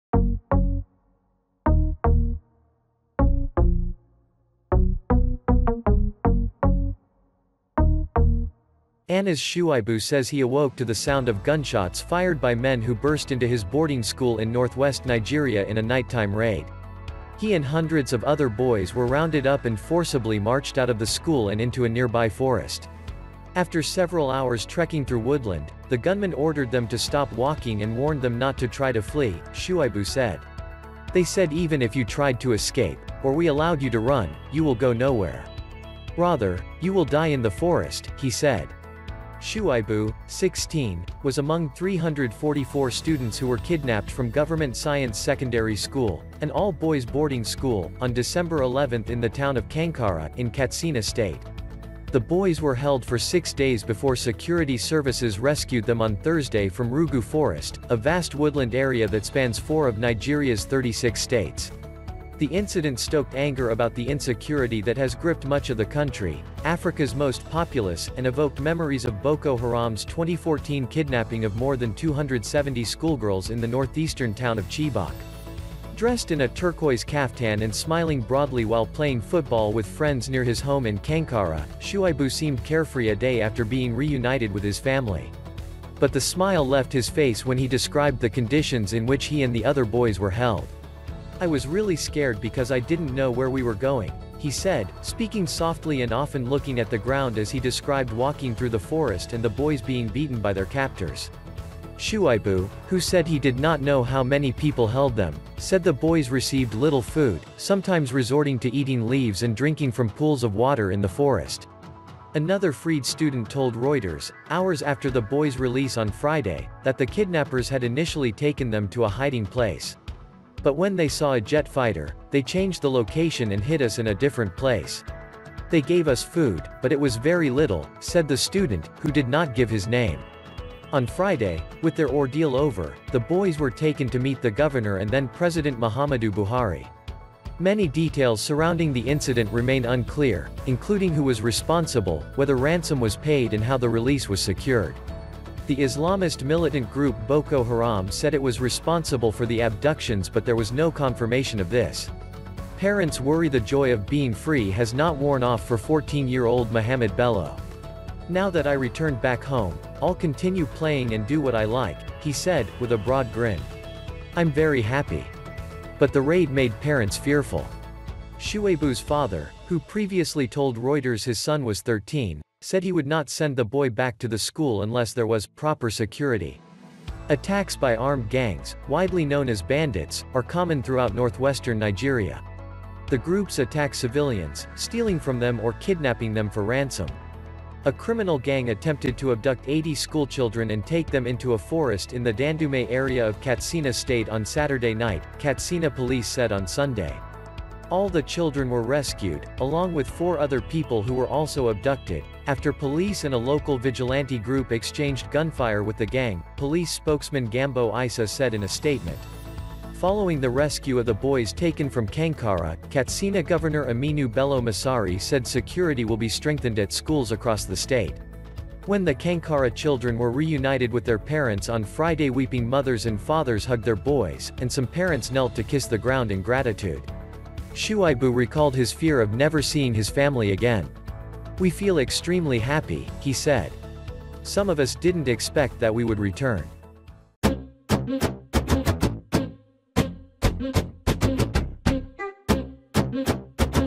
'You will die in the forest' Nigerian schoolboys describe kidnap ordeal
Annas Shuaibu says he awoke to the sound of gunshots fired by men who burst into his boarding school in northwest Nigeria in a nighttime raid. He and hundreds of other boys were rounded up and forcibly marched out of the school and into a nearby forest.

After several hours trekking through woodland, the gunmen ordered them to stop walking and warned them not to try to flee, Shuaibu said. “They said even if you tried to escape, or we allowed you to run, you will go nowhere. Rather, you will die in the forest,” he said.

Shuaibu, 16, was among 344 students who were kidnapped from Government Science Secondary School, an all-boys boarding school, on Dec. 11 in the town of Kankara, in Katsina state.

The boys were held for six days before security services rescued them on Thursday from Rugu forest, a vast woodland area that spans four of Nigeria’s 36 states.

The incident stoked anger about the insecurity that has gripped much of the country, Africa’s most populous, and evoked memories of Boko Haram’s 2014 kidnapping of more than 270 schoolgirls in the northeastern town of Chibok.

Dressed in a turquoise kaftan and smiling broadly while playing football with friends near his home in Kankara, Shuaibu seemed carefree a day after being reunited with his family.

But the smile left his face when he described the conditions in which he and the other boys were held.

“I was really scared because I didn’t know where we were going,” he said, speaking softly and often looking at the ground as he described walking through the forest and the boys being beaten by their captors.

Shuaibu, who said he did not know how many people held them, said the boys received little food, sometimes resorting to eating leaves and drinking from pools of water in the forest.

Another freed student told Reuters, hours after the boys’ release on Friday, that the kidnappers had initially taken them to a hiding place.

“But when they saw a jet fighter, they changed the location and hid us in a different place. They gave us food, but it was very little,” said the student, who did not give his name.

On Friday, with their ordeal over, the boys were taken to meet the governor and then President Muhammadu Buhari.

Many details surrounding the incident remain unclear, including who was responsible, whether ransom was paid and how the release was secured. The Islamist militant group Boko Haram said it was responsible for the abductions but there was no confirmation of this.

PARENTS WORRY
The joy of being free has not worn off for 14-year-old Muhammed Bello.

“Now that I returned back home, I’ll continue playing and do what I like,” he said, with a broad grin. “I’m very happy.”

But the raid made parents fearful.

Shuaibu’s father, who previously told Reuters his son was 13, said he would not send the boy back to the school unless there was “proper security”.

Attacks by armed gangs, widely known as bandits, are common throughout northwestern Nigeria. The groups attack civilians, stealing from them or kidnapping them for ransom.

A criminal gang attempted to abduct 80 schoolchildren and take them into a forest in the Dandume area of Katsina state on Saturday night, Katsina police said on Sunday.

All the children were rescued, along with four other people who were also abducted, after police and a local vigilante group exchanged gunfire with the gang, police spokesman Gambo Isah said in a statement.

Following the rescue of the boys taken from Kankara, Katsina Governor Aminu Bello Masari said security will be strengthened at schools across the state.

When the Kankara children were reunited with their parents on Friday weeping mothers and fathers hugged their boys, and some parents knelt to kiss the ground in gratitude.

Shuaibu recalled his fear of never seeing his family again.

“We feel extremely happy,” he said. “Some of us didn’t expect that we would return.”